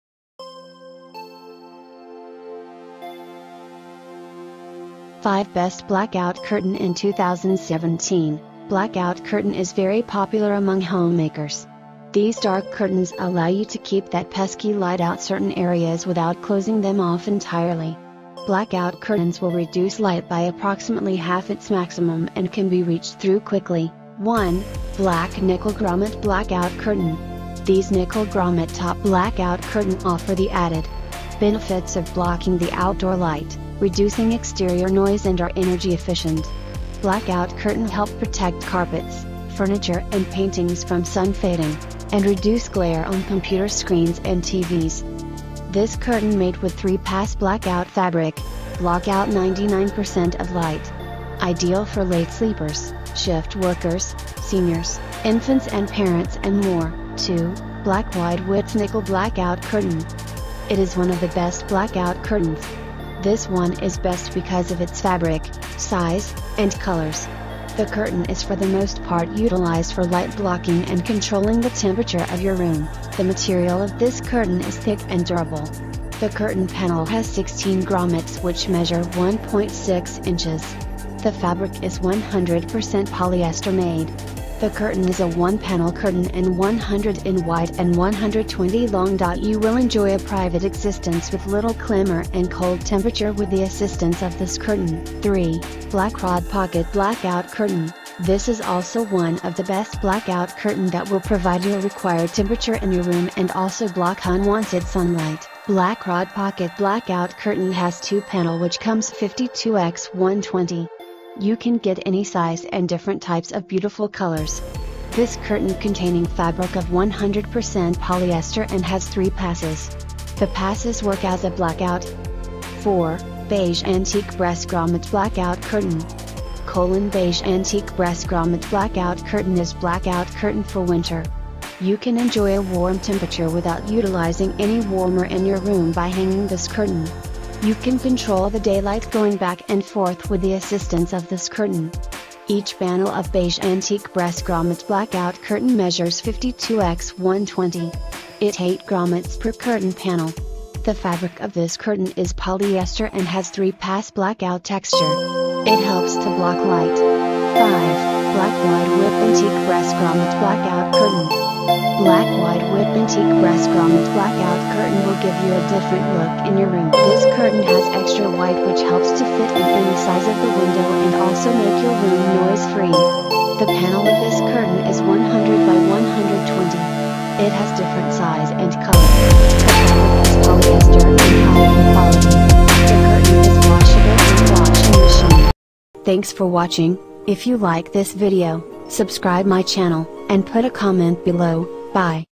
5 Best Blackout Curtain in 2017 | Blackout Curtain Review
5 Best Blackout Curtain in 2017 |  Blackout Curtain Review

Blackout Curtain is very popular among homemakers. These dark curtains allow you to keep that pesky light out certain areas without closing them off entirely. Blackout curtains will reduce light by approximately half its maximum and can be reached through quickly.

5 Best Blackout Curtain:

1/ Black Nickel Grommet Blackout Curtain: These nickel grommet top blackout curtain offer the added.
Benefits of blocking the outdoor light, reducing exterior noise and are energy efficient. Blackout curtain
Help protect carpets, furniture and paintings from sun fading, and reduce glare on computer screens and TVs.

This Curtain Made with three pass blackout fabric, block out 99% of light. Ideal for late sleepers, shift workers, seniors, infants and parents and more

2/ Black Wide Width Nickel Blackout Curtain: It is one of the best blackout curtains. This one is best because of its fabric, size, and colors. The curtain is for the most part utilized for light blocking and controlling the temperature of your room.

The material of this curtain is thick and durable. The curtain panel has 16 grommets which measure 1.6 inches. The fabric is 100% polyester made. The curtain is a one-panel curtain and 100” in wide and 120” long.You will enjoy a private existence with little clamor and cold temperature with the assistance of this curtain.

3/ Black Rod Pocket Blackout Curtain: This is also one of the Best Blackout Curtain that will provide your required temperature in your room and also block unwanted sunlight.
Black Rod Pocket Blackout Curtain has two panel which comes 52”x120”.You can get any size and different types of beautiful colors. This curtain containing fabric of 100% polyester and has three passes. The passes work as a blackout.

4/ Beige Antique Brass Grommet Blackout Curtain: Beige Antique Brass Grommet Blackout Curtain is blackout curtain for winter. You can enjoy a warm temperature without utilizing any warmer in your room by hanging this curtain. You can control the daylight going back and forth with the assistance of this curtain.

Each panel of Beige Antique Brass Grommet Blackout Curtain measures 52”x120”.It eight grommets per curtain panel. The fabric of this curtain is polyester and has three pass blackout texture. It helps to block light.

5/ Black Wide Width Antique Brass Grommet Blackout Curtain: Black Wide Width Antique Brass Grommet Blackout Curtain will give you a different look in your room.
This curtain has extra wide which helps to fit in any size of the window and also make your room noise free. The panel of this curtain is 100”x 120”.It has different size and color. The fabric is polyester and high in quality. The curtain is washable in washing machine.